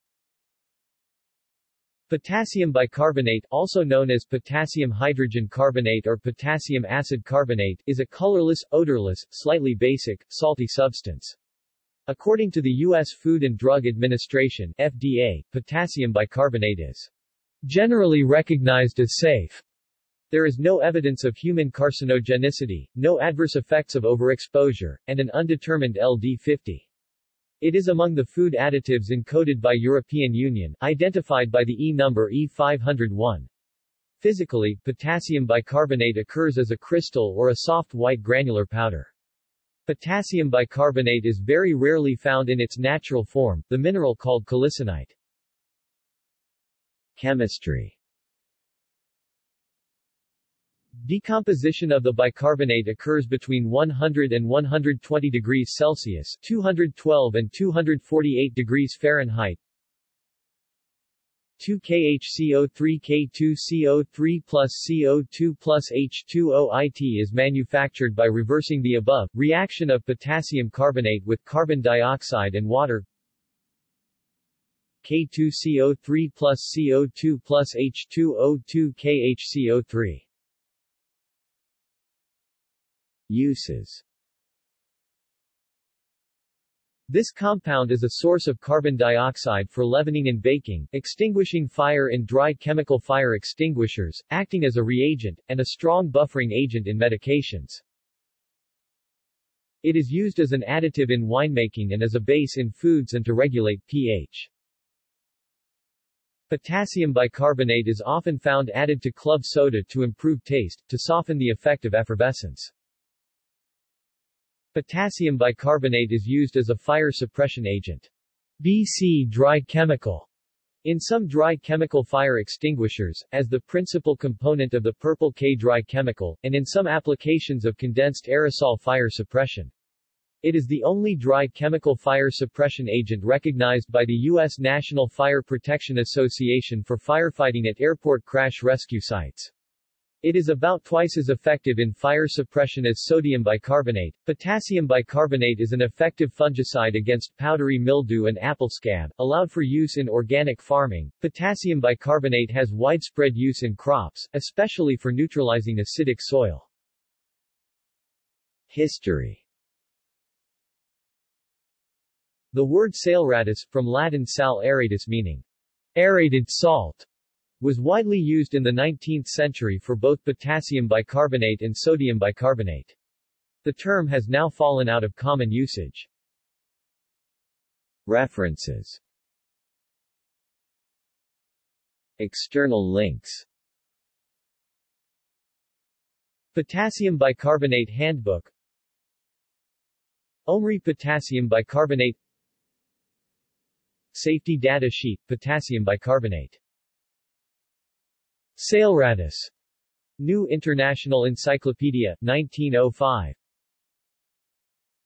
Potassium bicarbonate | Wikipedia audio article
This is an audio version of the Wikipedia Article:
Potassium bicarbonate

SUMMARY
Potassium bicarbonate (also known as potassium hydrogen carbonate or potassium acid carbonate) is a colorless, odorless, slightly basic, salty substance. According to the U.S. Food and Drug Administration (FDA), potassium bicarbonate is "generally recognized as safe".  There is no evidence of human carcinogenicity, no adverse effects of overexposure, and an undetermined LD50. It is among the food additives encoded by European Union, identified by the E number E501. Physically, potassium bicarbonate occurs as a crystal or a soft white granular powder. Potassium bicarbonate is very rarely found in its natural form, the mineral called kalicinite.